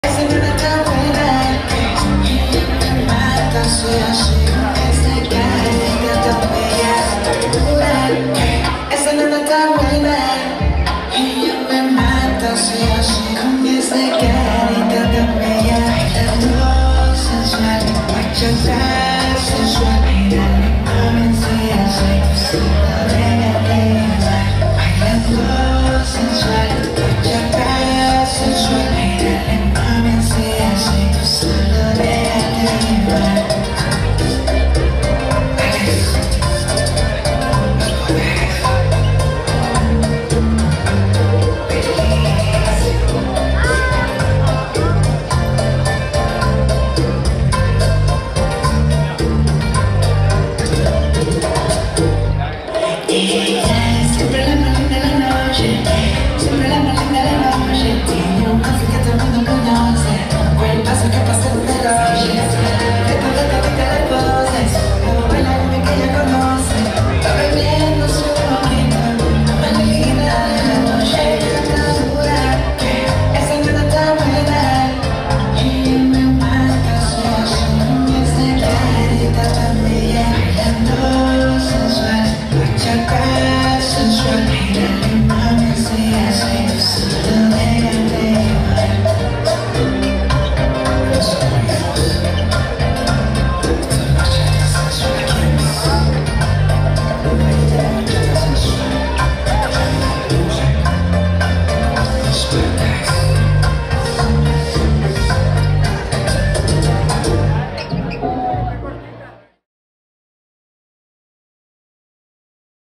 Korke Maestro Bachata Sensual at timbachata | by Dance Vida
Un placer verlo bailar al maestro de la bachata